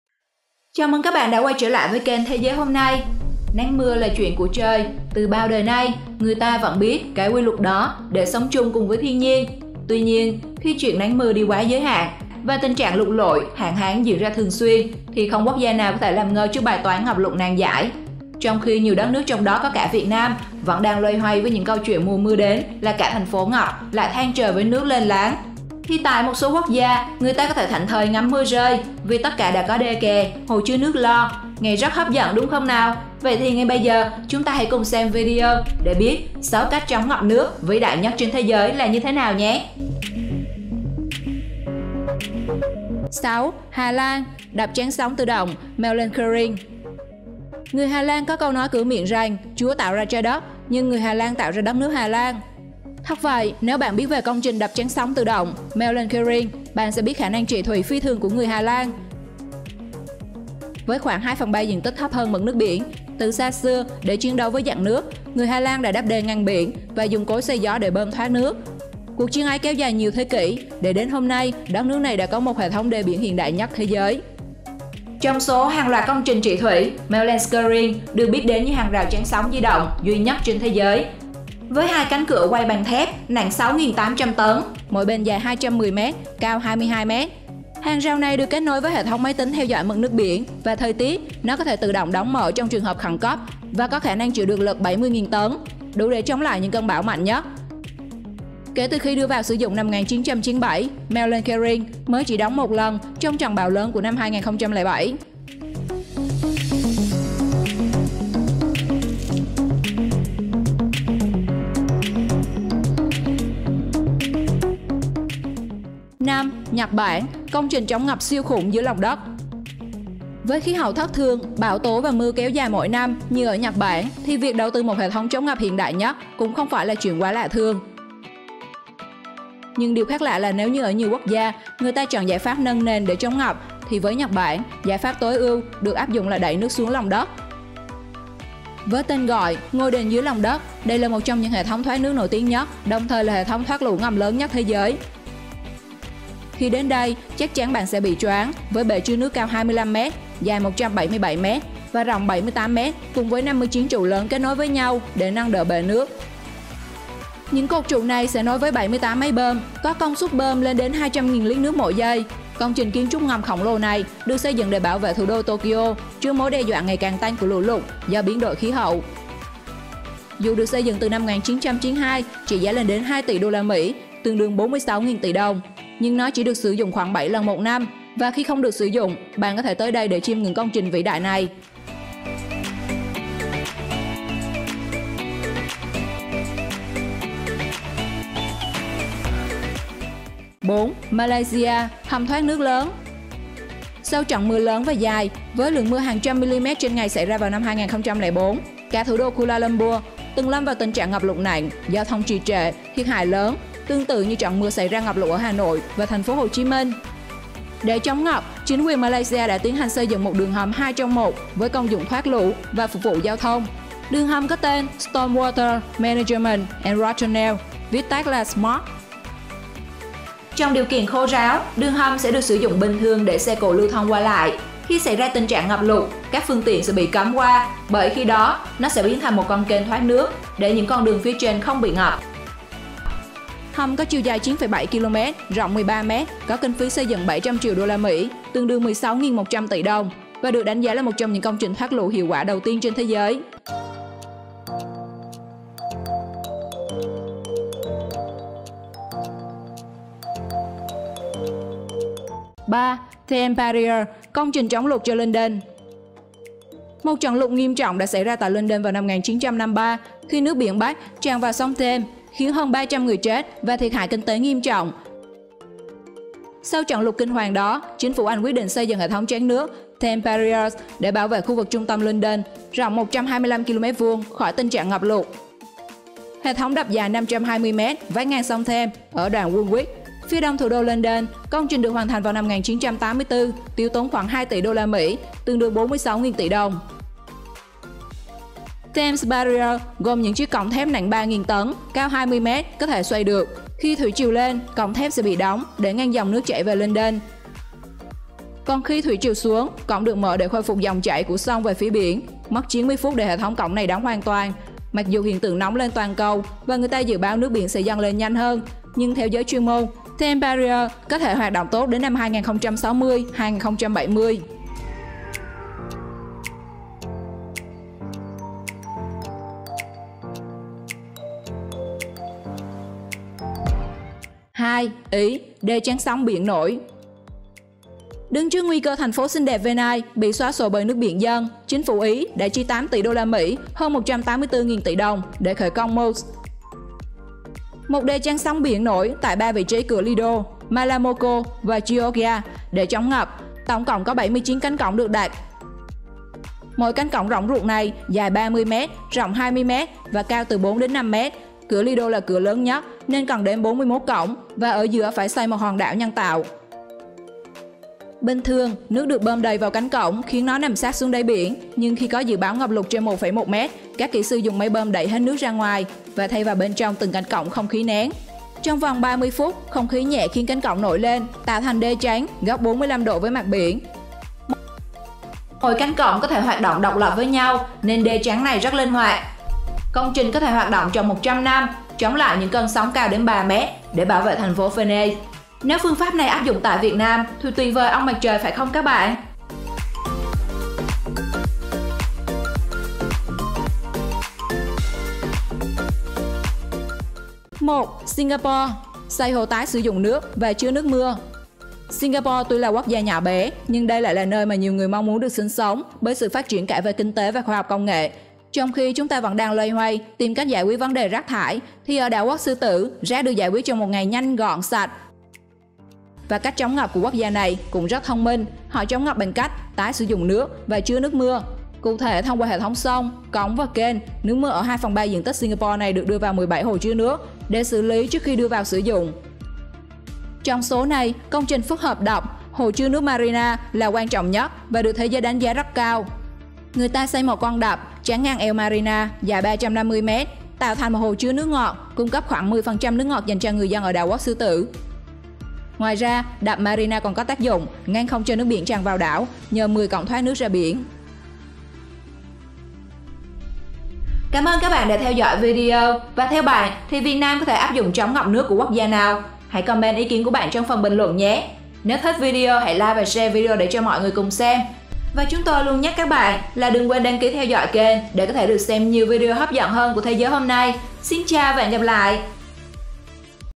5 Cách Chống Ngập Nước Vĩ Đại Nhất Trên Thế Giới
5 Cách Chống Ngập Nước Vĩ Đại Nhất Trên Thế Giới

Xin chào các bạn, Nắng mưa là chuyện của trời; từ bao đời này, người ta vẫn biết cái quy luật đó để sống chung cùng thiên nhiên. Tuy nhiên, khi chuyện nắng mưa đi quá giới hạn và tình trạng lụt lội, hạn hán diễn ra thường xuyên thì không quốc gia nào có thể làm ngơ trước bài toán ngập lụt nan giải. Trong khi nhiều đất nước trong đó có cả Việt Nam vẫn đang loay hoay với những câu chuyện mùa mưa đến là cả thành phố lại than trời vì nước ngập lênh láng, thì tại một số quốc gia, người ta có thể thảnh thơi ngắm mưa rơi vì tất cả đã có đê kè, hồ chứa nước lo. Nghe rất hấp dẫn đúng không nào? vậy thì ngay bây giờ chúng ta hãy cùng xem video hôm nay để biết 6 Cách Chống Ngập Nước Vĩ Đại Nhất Trên Thế Giới là như thế nào nhé.

6. Hà Lan – đập chắn sóng tự động Maeslantkering
5. Nhật Bản – công trình chống ngập"siêu khủng" dưới lồng đất
4. Malaysia - Hầm thoát nước lớn
3. Thames Barrier, công trình chống lụt cho London
2. Ý - đê chắn sóng biển nổi
1. Singapore – xây hồ tái sử dụng nước và chứa nước mưa